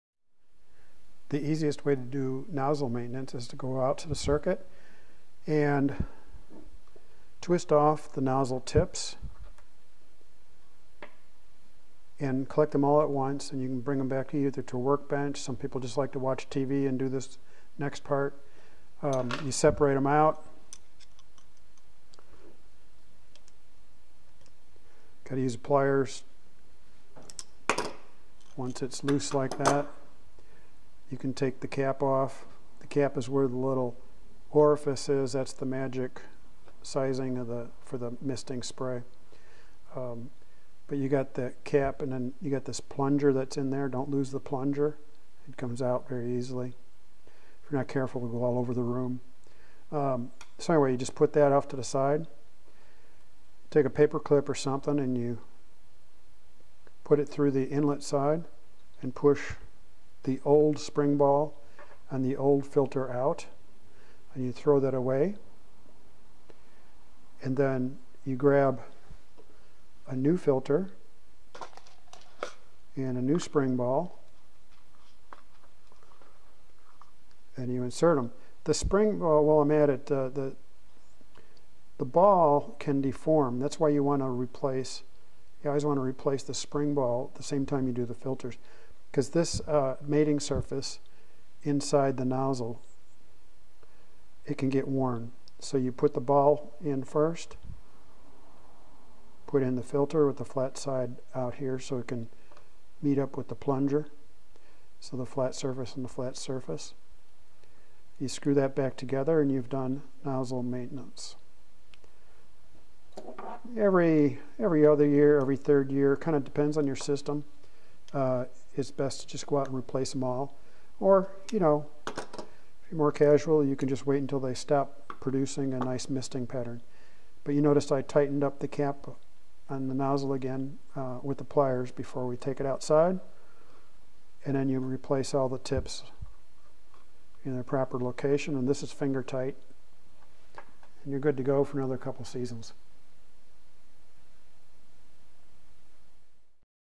Mistaway Slimline Nozzle Maintenance for the ORIGINAL, pre-2015, nozzle design
MistAway nozzle maintenance or repair parts for misting systems.  MistAway misting system nozzle maintenance & other items.

Every few years it's best to replace the nozzle internal parts or the nozzle tips to produce an optimum misting pattern.  This video shows how to replace those parts for the ORIGINAL (pre-2015) Slimline nozzle design.

Automatic Mosquito Control by Greenville Mosquito, Inc.
8595 Pelham Rd. Ste 400
Greenville, South Carolina 29615